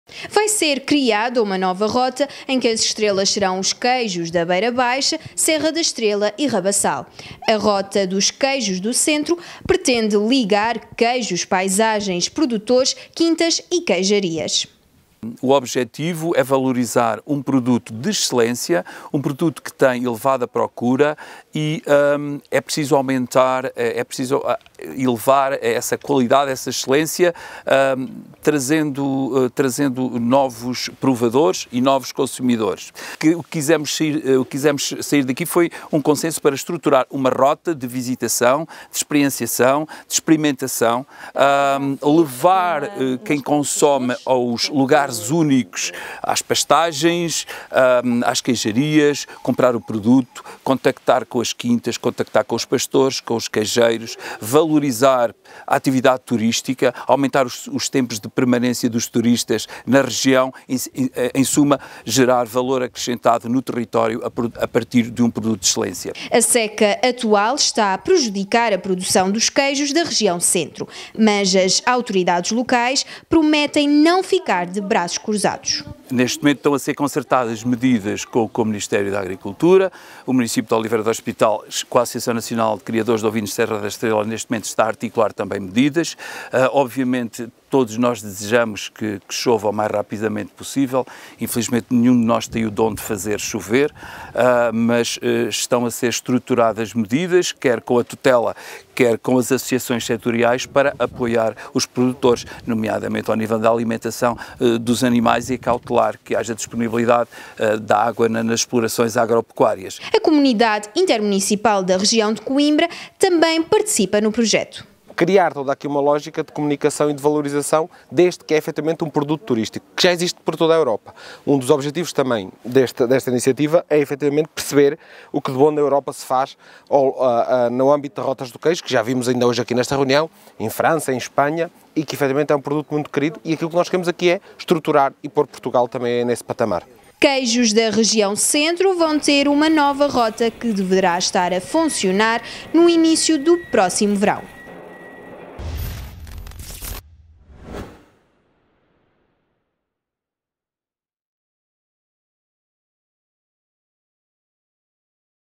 Queijos Serra da Estrela, Beira Baixa e do Rabaçal vão fazer parte de Rota turística
A CentroTV é uma webtv com sede na região Centro de informação just in time.

Sempre no Centro dos Acontecimentos!